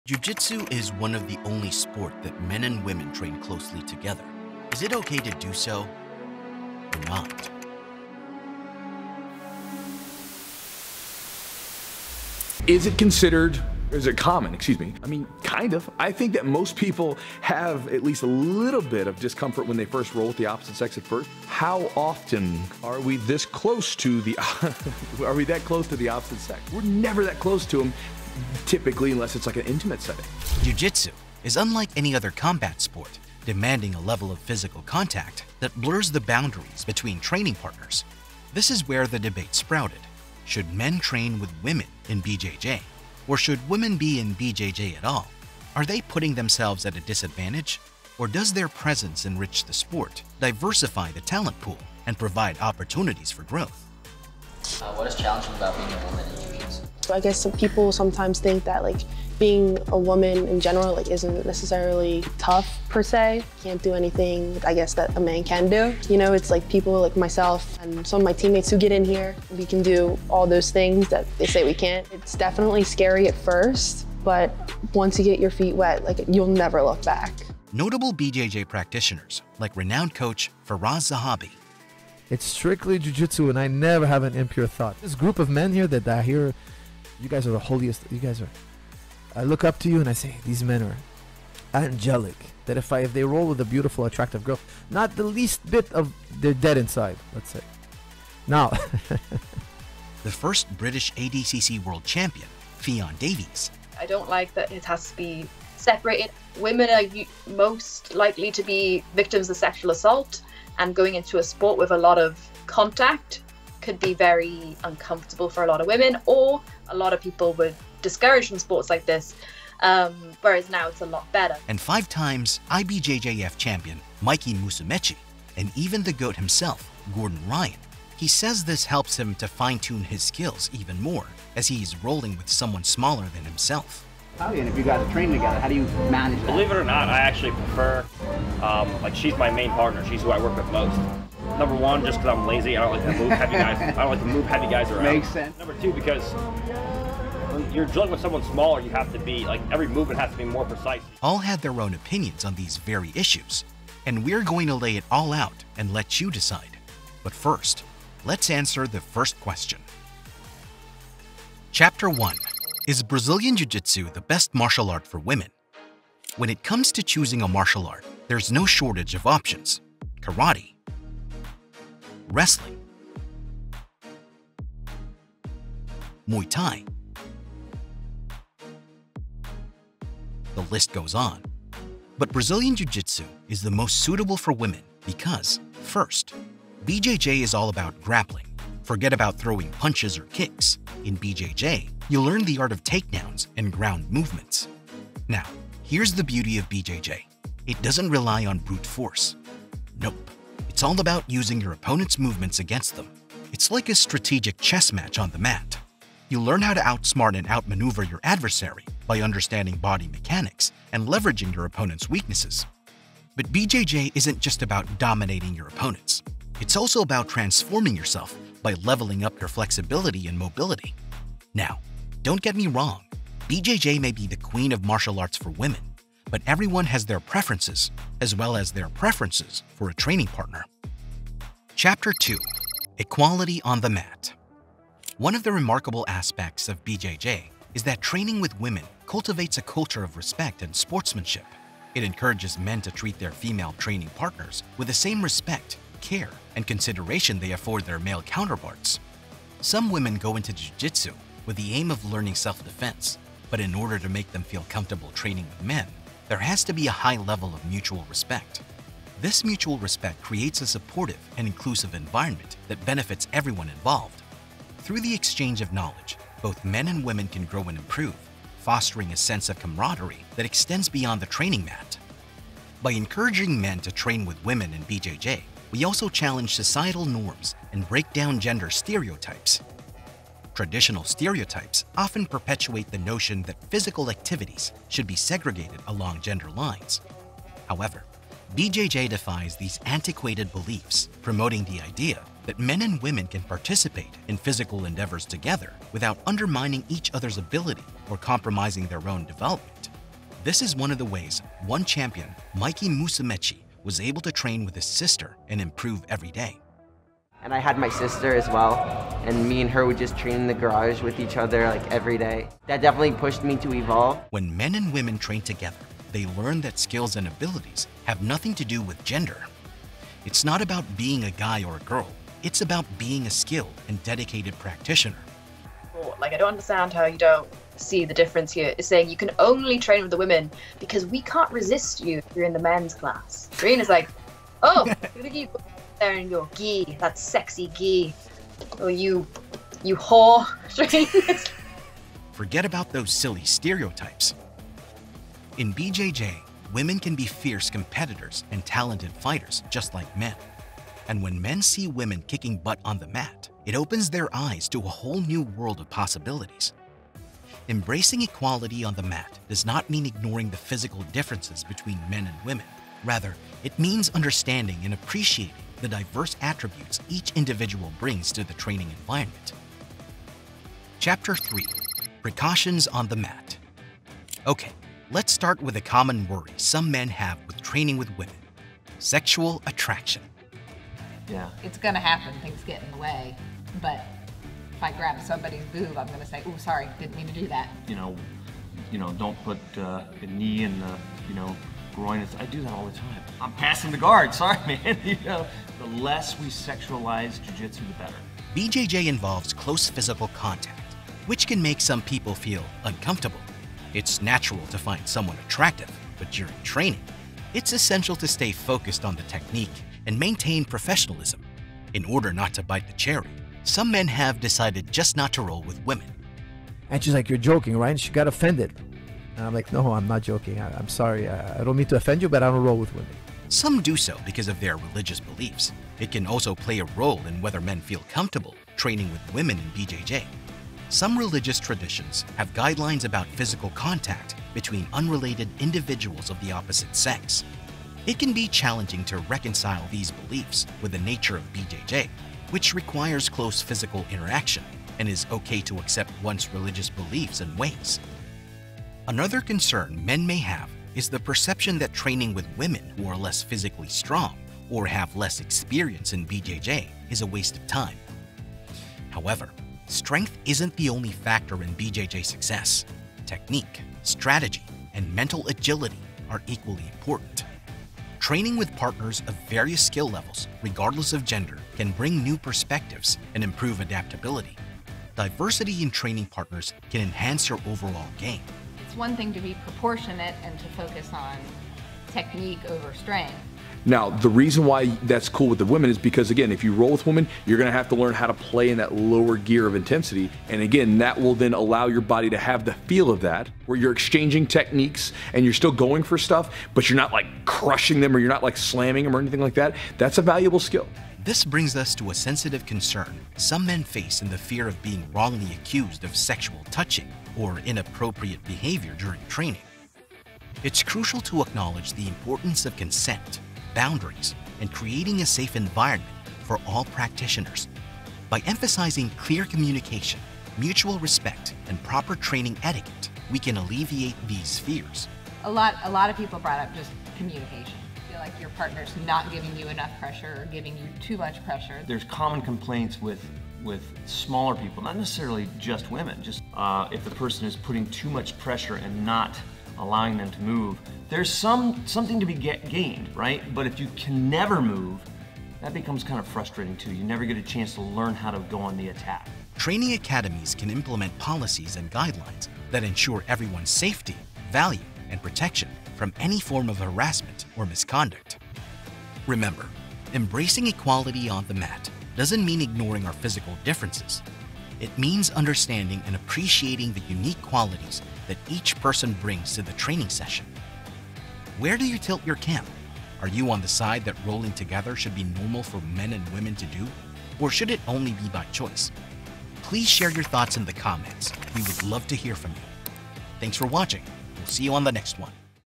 Untold TRUTH Behind Boys Grappling Girls
Jiu Jitsu is unlike any other combat sport, demanding a level of physical contact that blurs the boundaries between training partners.

This is where the debate sprouted: should men train with women be in BJJ? Or Should Women be in BJJ at all? Are they putting themselves at a disadvantage? Or does their presence enrich the sport, diversify the talent pool, and provide opportunities for growth?

Notable BJJ practitioners, like renowned coach Firas Zahabi, the first British ADCC world champion, Ffion Davies, and 5 times IBJJF champion Mikey Musumeci,and even the GOAT himself, Gordon Ryan all had their own opinions on these very issues, and we’re going to lay it all out and let you decide.

But first, let’s answer the first question.